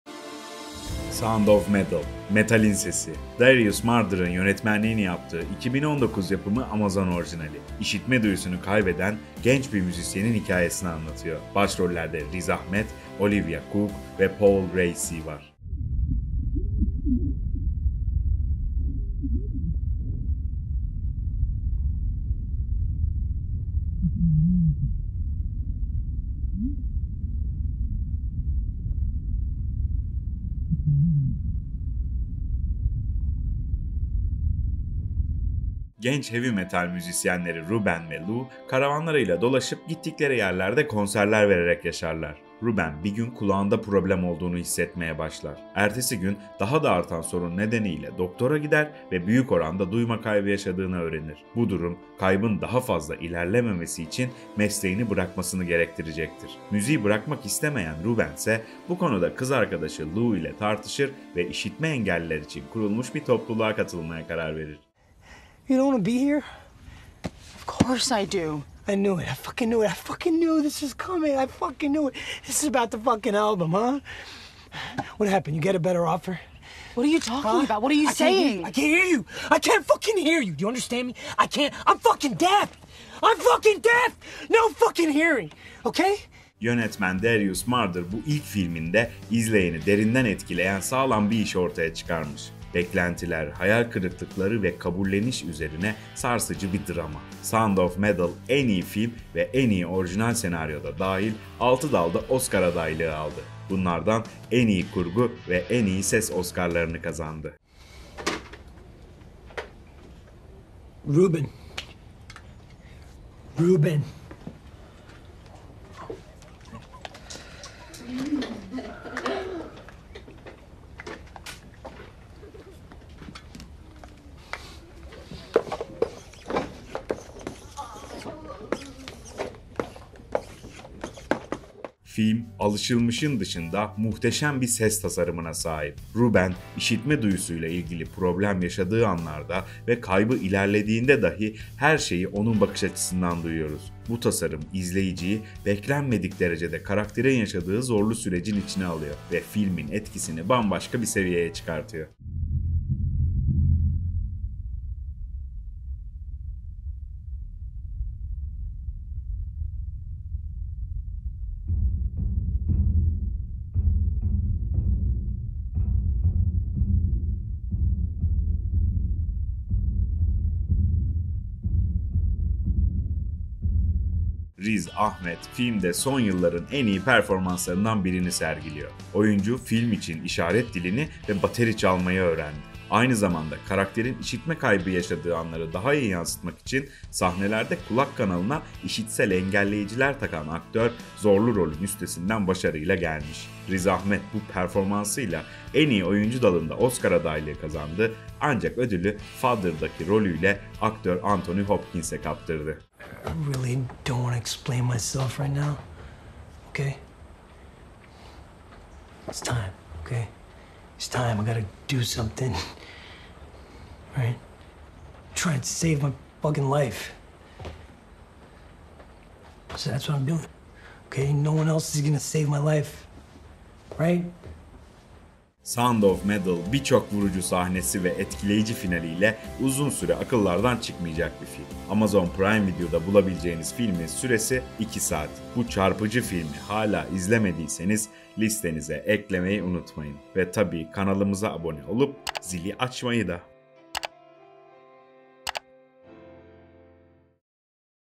Amazon Prime Video Film Tavsiyesi: Sound Of Metal - Metalin Sesi
Sound of Metal, (Metalin Sesi) Darius Marder'in yönetmenliğini yaptığı 2019 yapımı Amazon orijinali. İşitme duyusunu kaybeden genç bir müzisyeninin hikayesini anlatan filmin başrollerinde Riz Ahmed, Olivia Cooke ve Paul Raci var. Alışılmışın dışında muhteşem ses tasarımıyla, unutulmaz oyunculuklarıyla, bir çok vurucu sahnesi ve etkileyici finaliyle son yılların en çarpıcı filmlerinden biri. Altı Oscar adaylığı kazanmış Sound of Metal'i Amazon Prime Video'da izleyebilirsiniz.